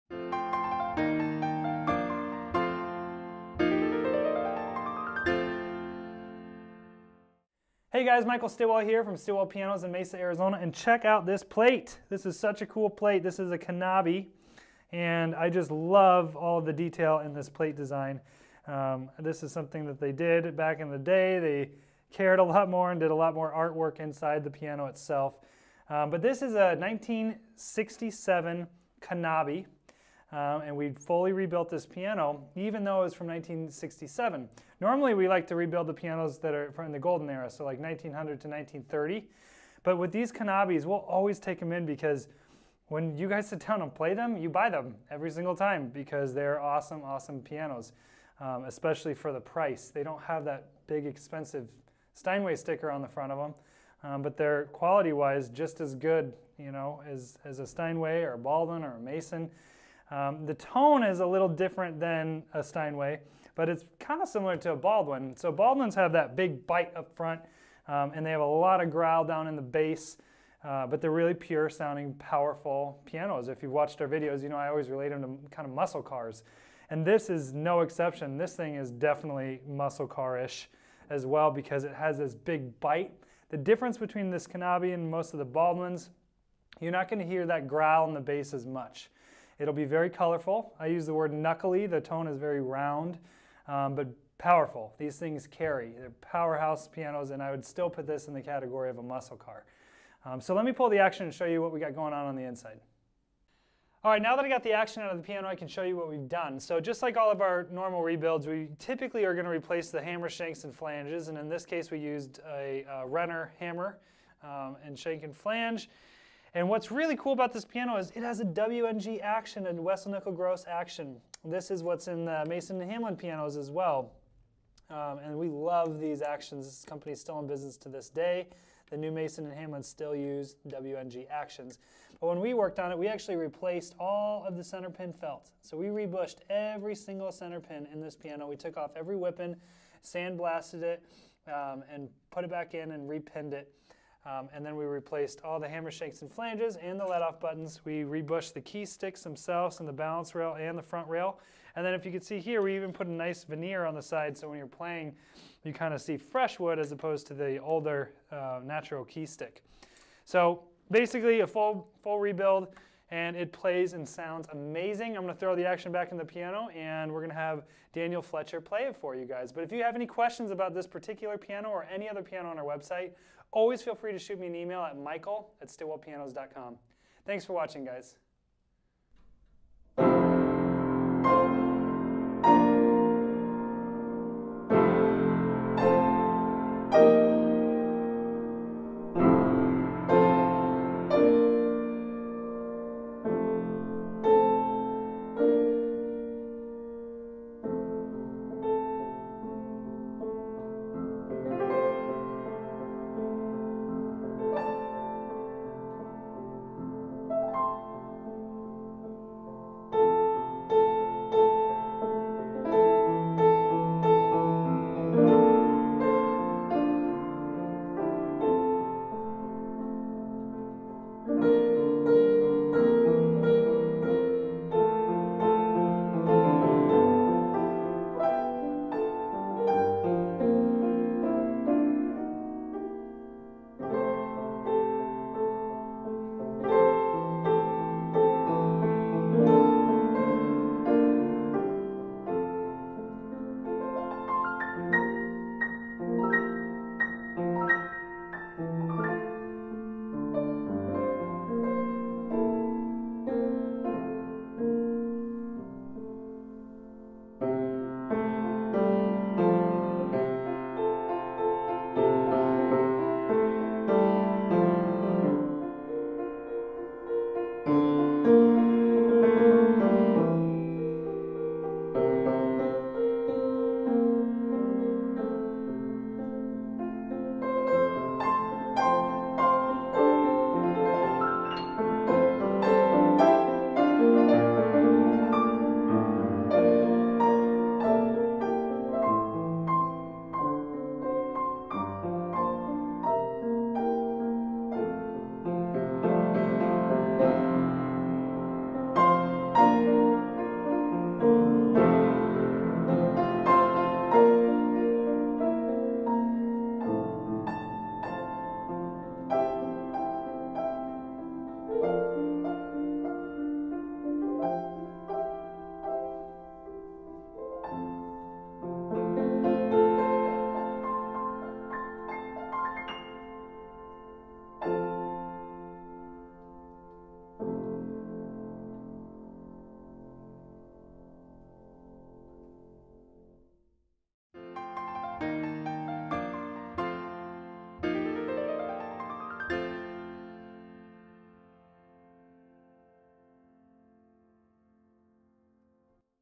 Antique Piano Knabe Parlor Grand | Built in 1897
This is an Antique Piano, a Knabe Parlor Grand that was built in 1897.
It is a wonderful classic piano that has this kind of exceptional features, which can only be exist in antique pianos.

Michael Stilwell will discuss this Knabe antique piano with details you will love to hear about this outstanding music instrument.

00:00-00:14 Introduction
00:14-01:42 Piano Rebuild Info
01:42-02:57 Inside the Piano
02:57-05:47 Piano Demonstration by Daniel Fletcher